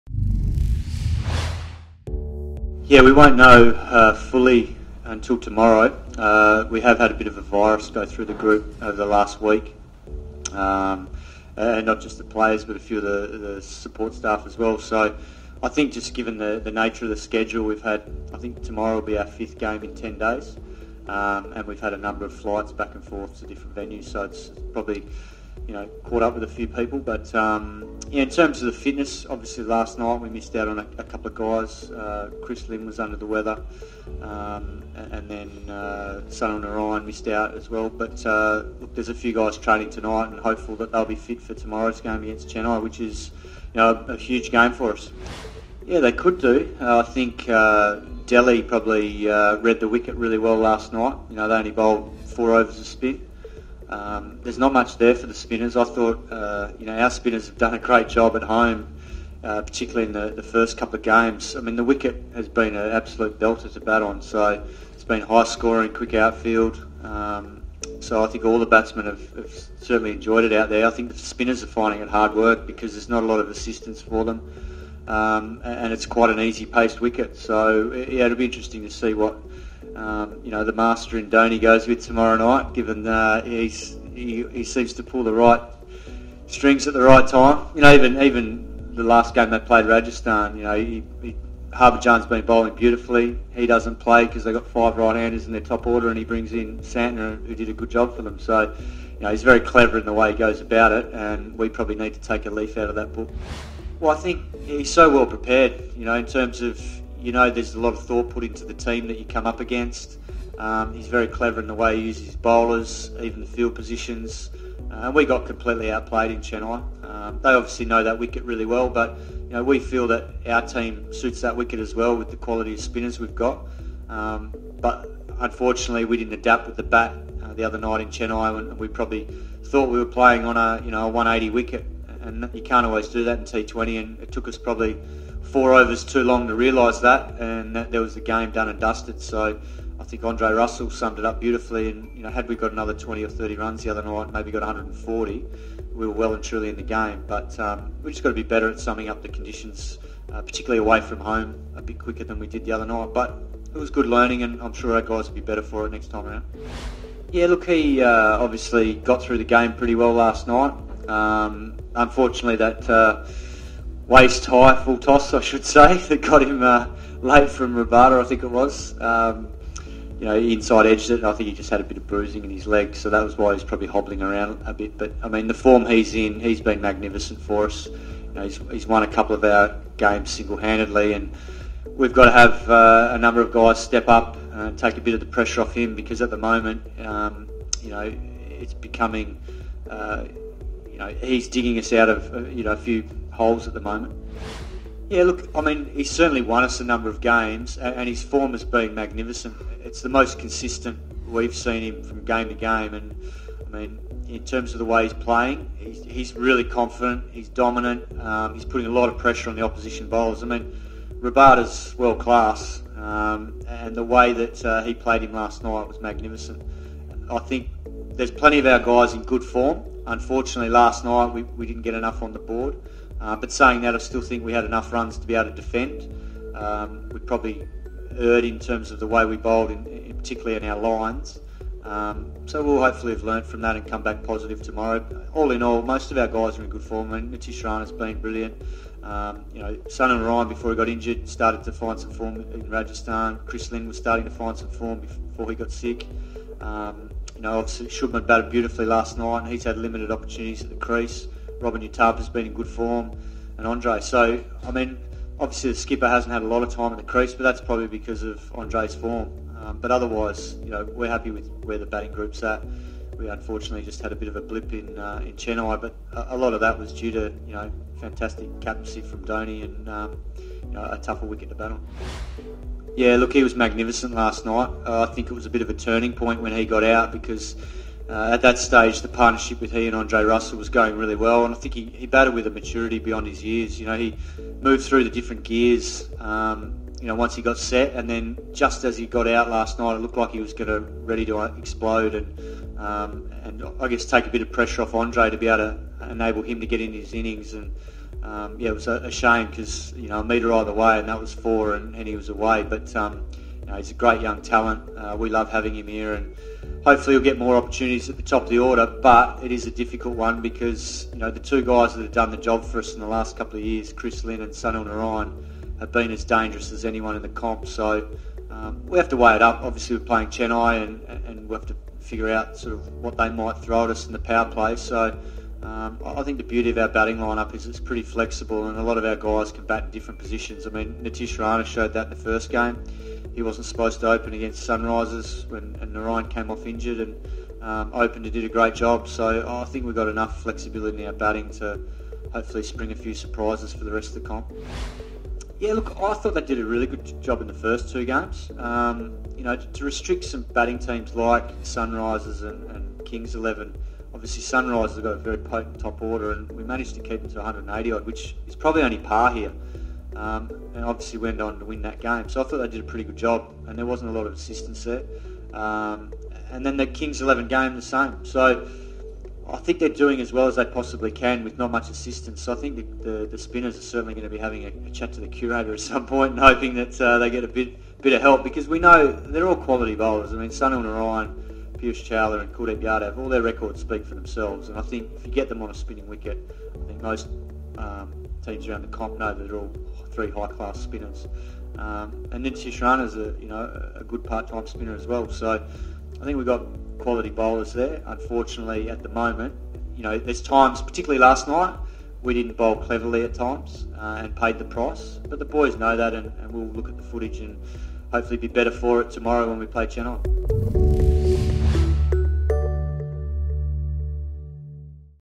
Dhoni seems to pull the right strings at the right time - Simon Katich
While praising MS Dhoni ahead of his side’s home encounter against Chennai, assistant coach Simon Katich dwelled upon Kolkata's reliance on Andre Russell and their spinners’ struggles on the Eden surface.